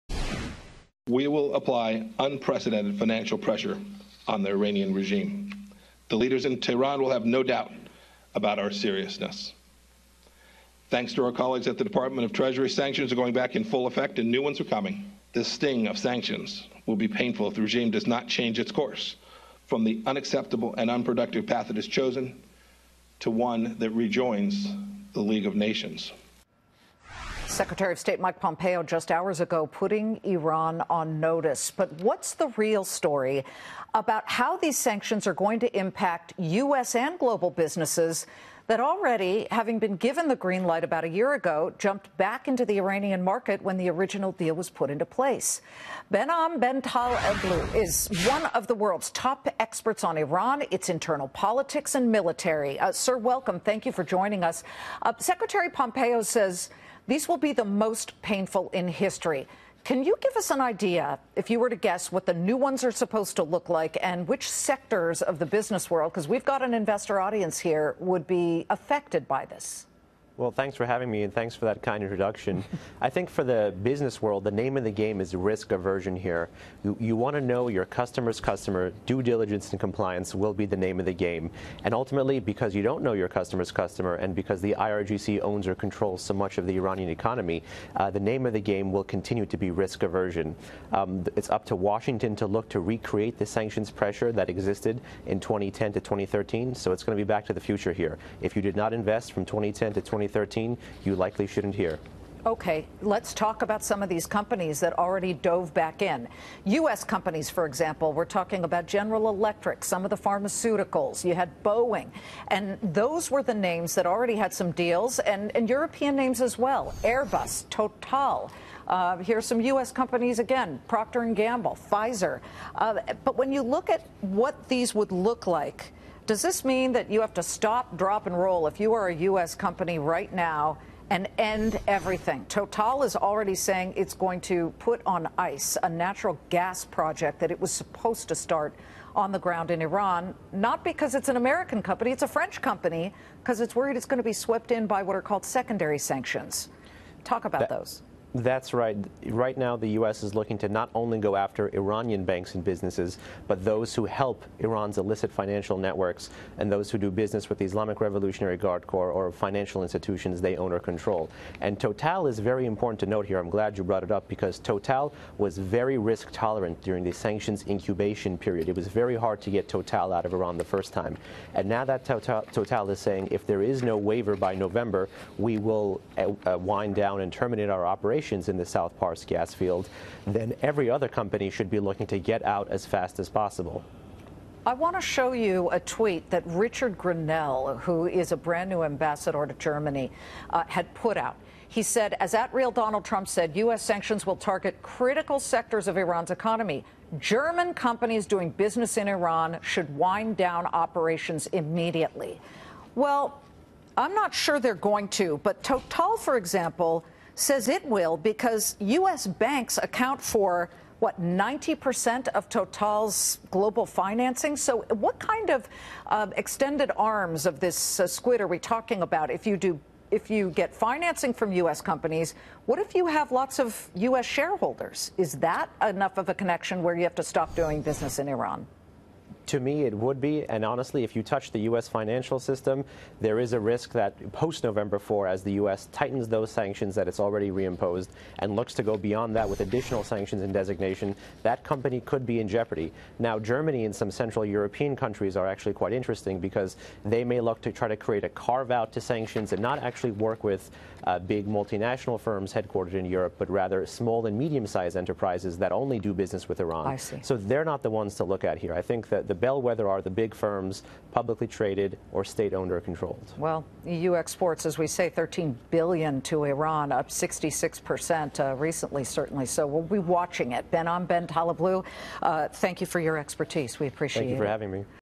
Pompeo’s sanctions threat may hurt US businesses
Foundation for Defense of Democracies senior Iran analyst Behnam Ben Taleblu on Secretary of State Mike Pompeo’s threat to implement stronger sanctions against Iran.